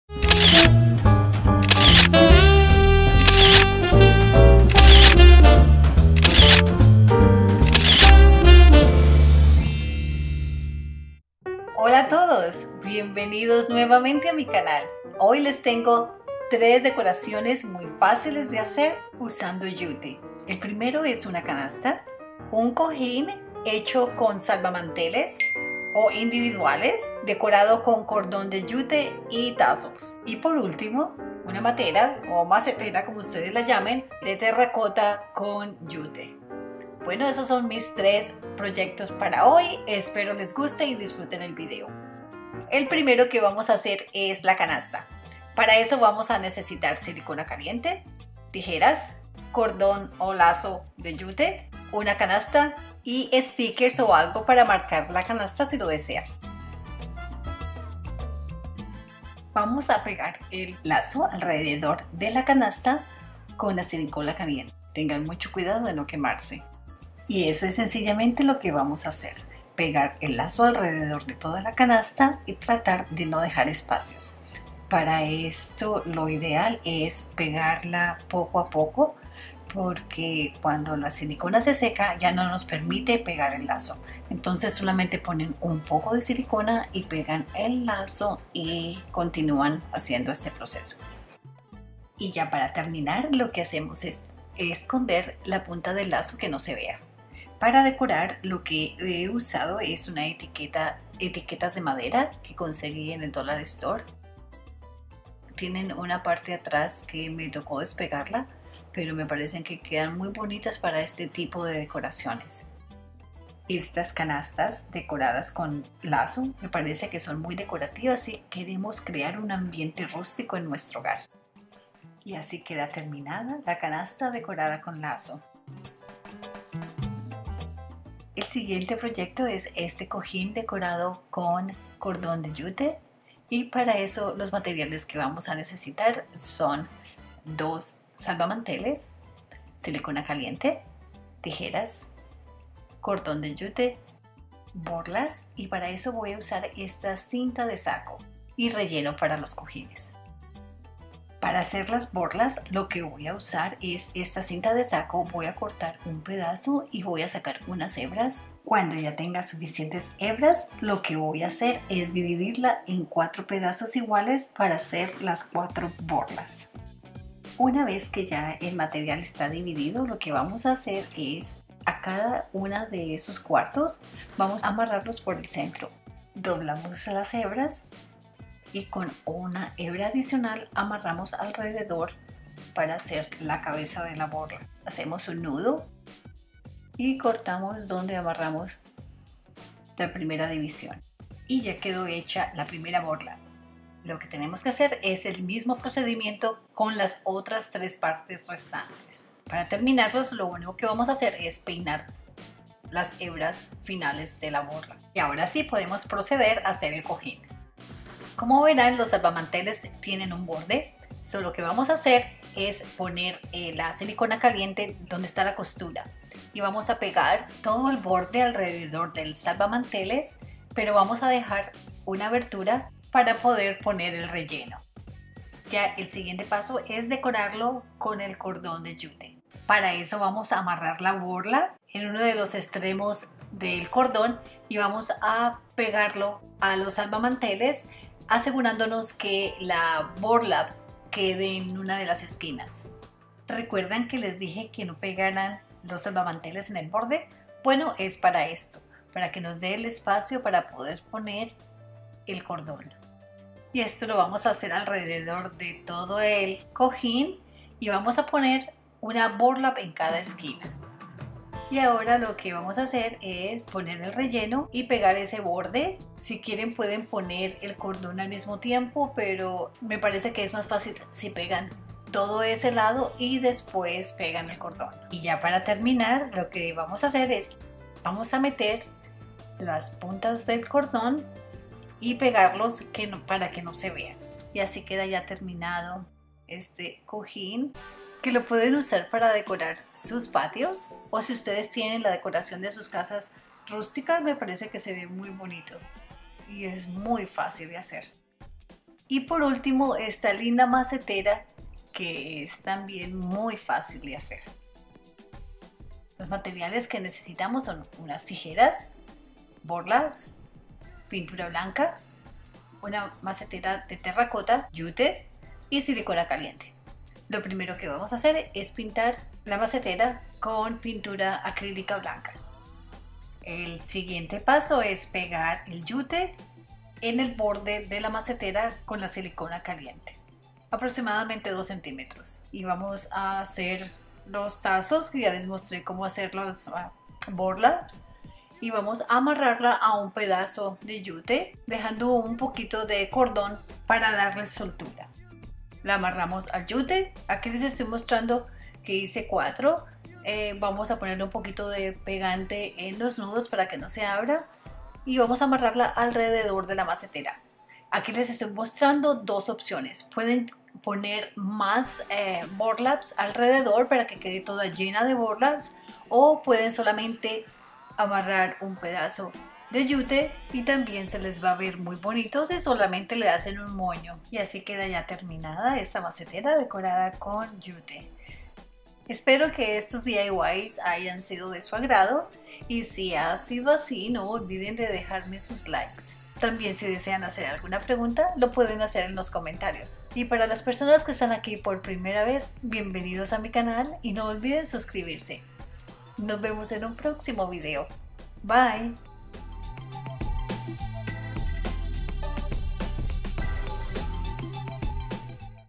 DECORACIONES FACILES CON YUTE
Hola todos😄 en este video les mostrare como hacer unas decoraciones fáciles y prácticas  para el hogar usando cordón de yute.